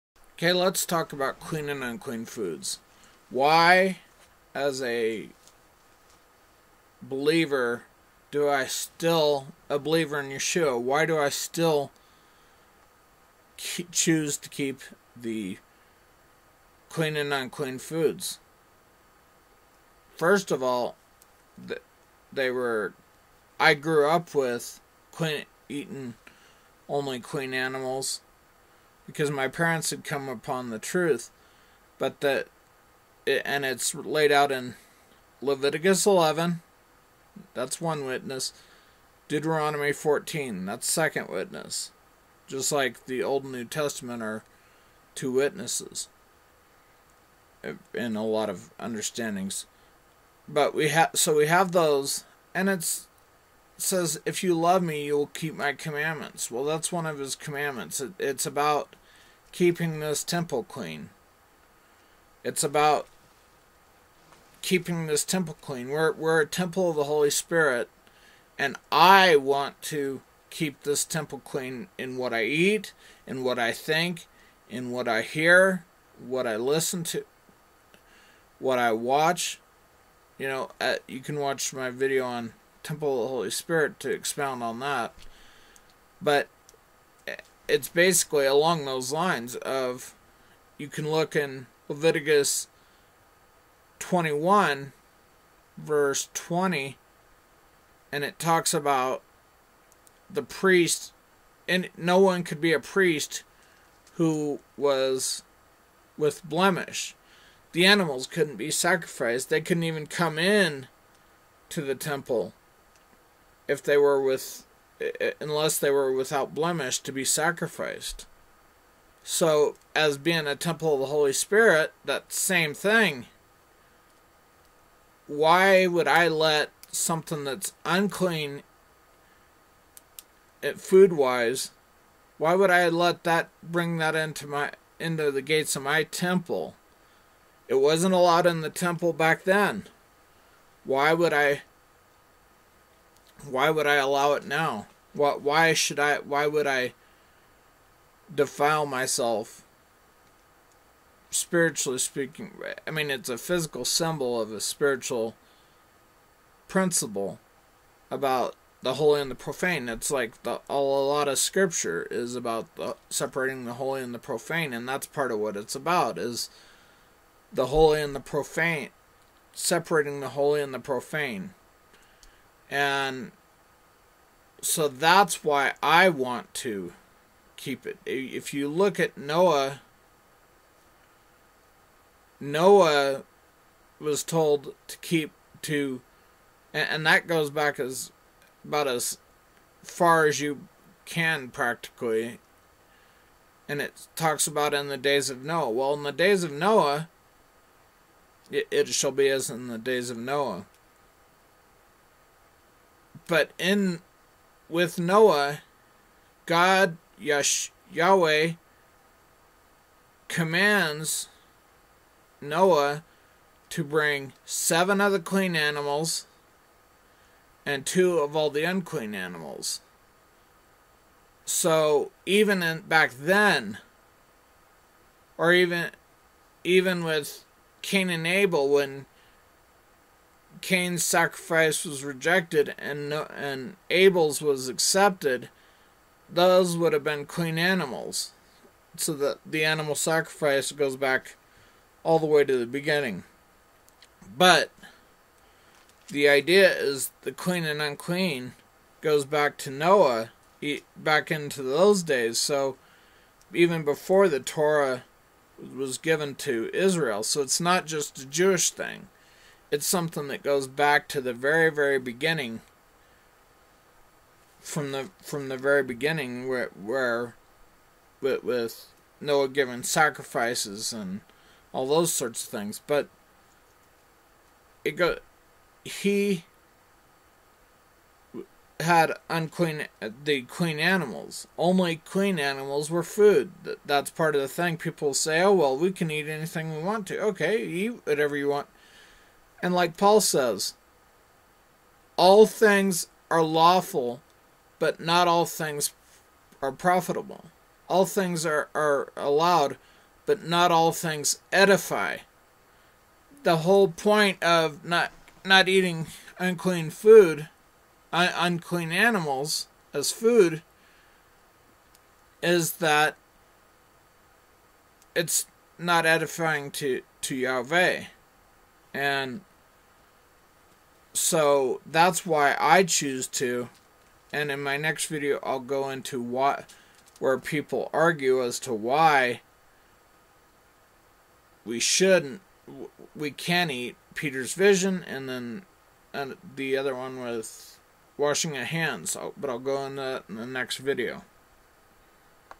Why I still follow dietary laws.
The "dietary laws" were never done away with,  the concept of clean animals goes back to Noah.  Genesis 7:2